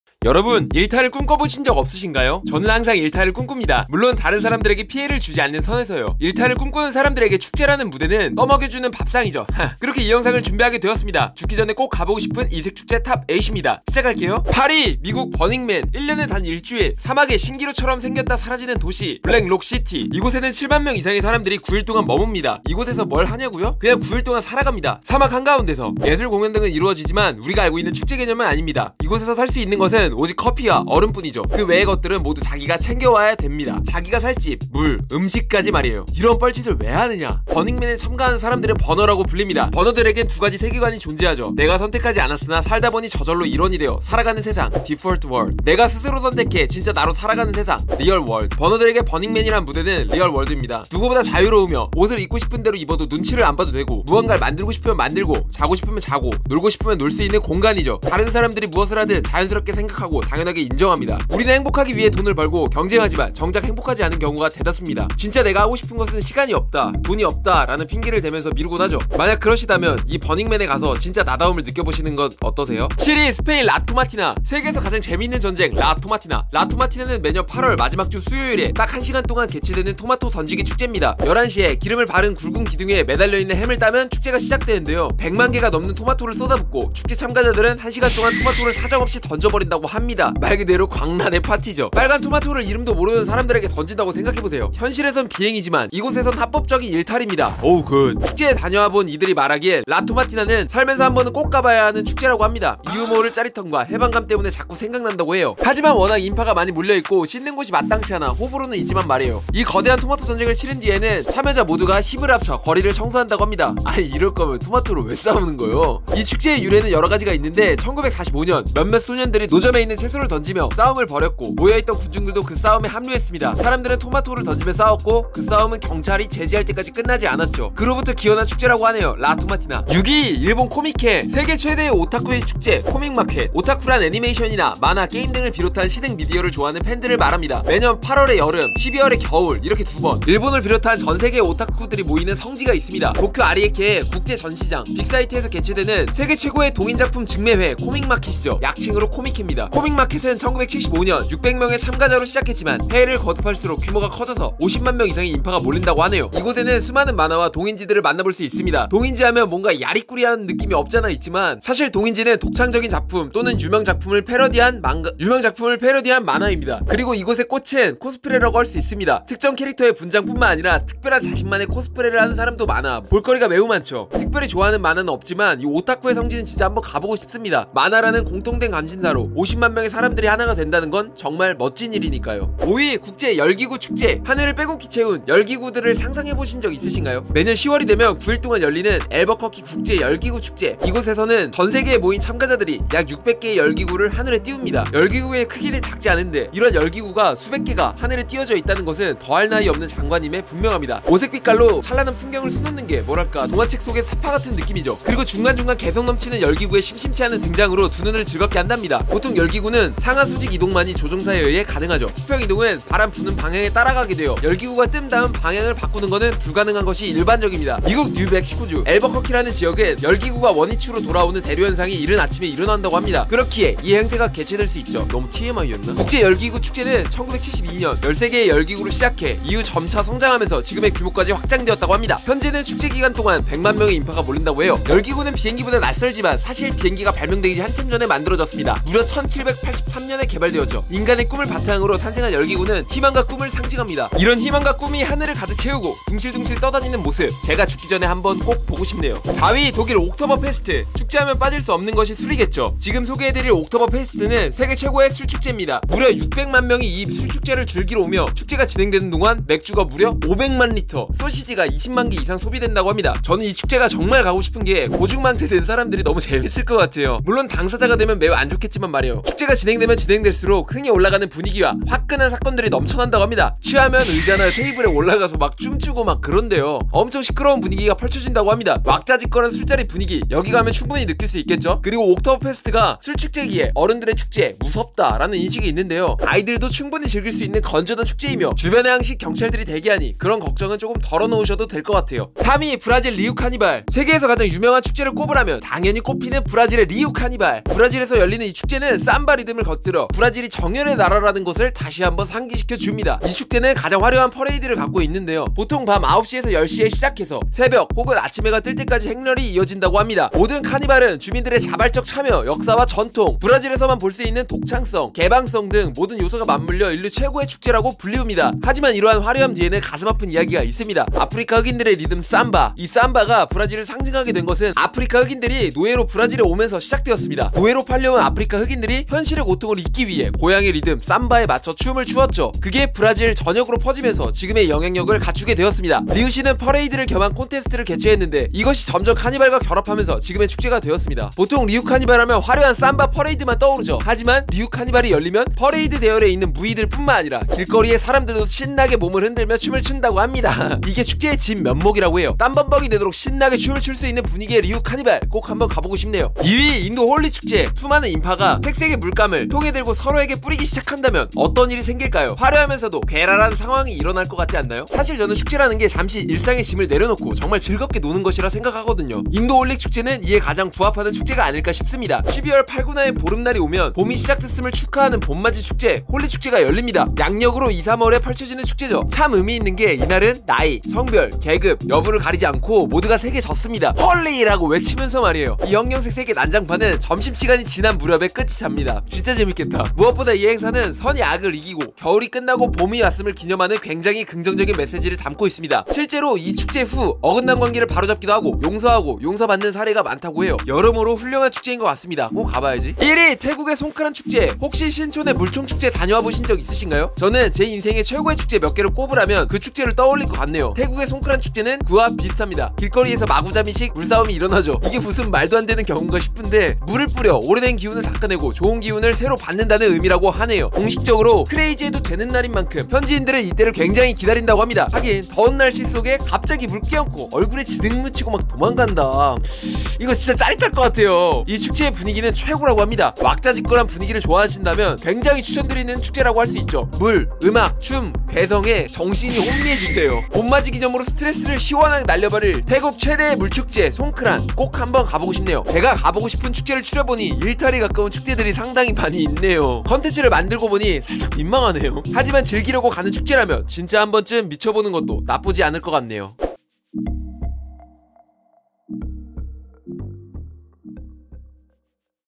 죽기전에 꼭 가보고 싶은 세계의 이색 축제 Top 8 !!
안녕하세요 차트로 말하는 남자 차말남입니다 : )
오늘은 요청을 받아왔던 세계의 이색축제에 대한 내용을 영상으로 만들어봤어요 !
항상 영상 재미있게 봐주셔서 감사드립니다 ~~!